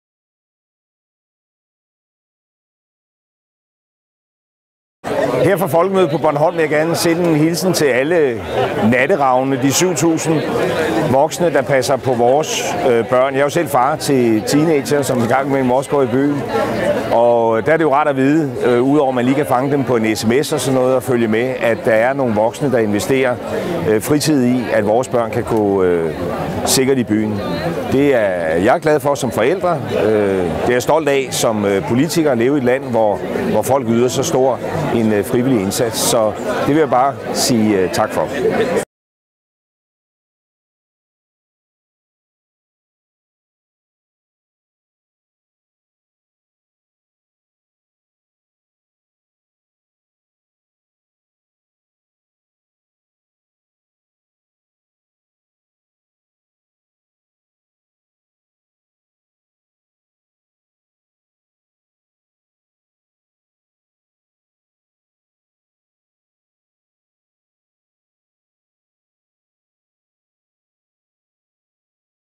Rart at voksne mennesker investerer fritid for vores børns tryghed.
Natteravne gør unge og deres forældre trygge ved nattelivet
Mere end ni ud af ti forældre kender Natteravnene, og både de unge og deres forældre føler sig mere trygge, når Natteravnene går ture i byens natteliv. Det viser rapport fra KORA.
I undersøgelsen er både de unge og deres forældre blevet bedt om at sammenligne tryghedsværdien ved Natteravnene med værdien af andre elementer i nattelivet. Og her viser det sig, at de unge tillægger Natteravnene lige så stor værdi, som hvis de havde en ekstra ven med i byen.
- Piger og mødre føler, at Natteravnene betyder mere for trygheden, end drenge og fædre gør. Vi kan også se, at forældre til piger generelt bliver mere trygge af at have Natteravnene i gadebilledet, end forældre til drenge gør, siger seniorforsker i KORA, Jacob Ladenburg, som har gennemført tryghedsanalyserne.